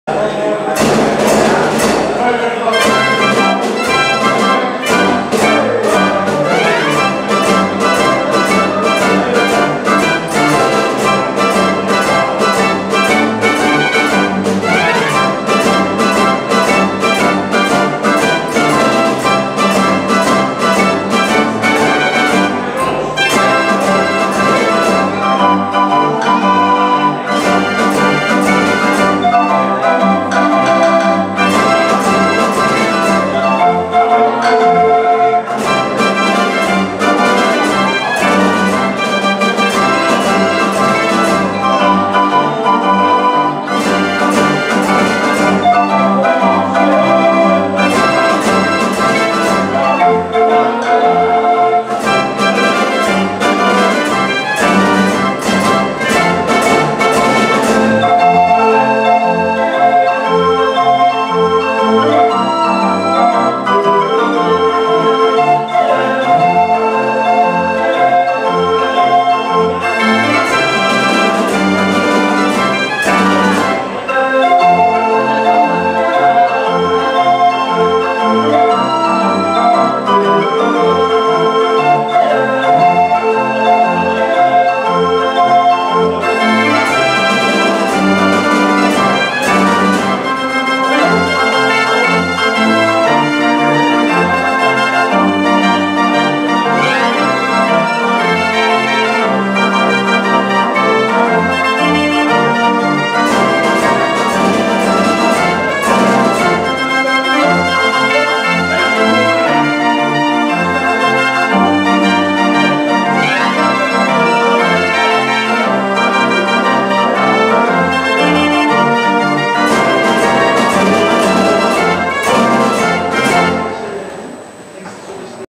SKO HL #03 25-07-21 Variant 52-Toets
Gastorgel: Variant Minning Gamma
Een 52-Toets orgel, gebouwd in 2000 door Gerard Roos.
Eigenaar: Peter Mink
Veel plezier, Wim & Greta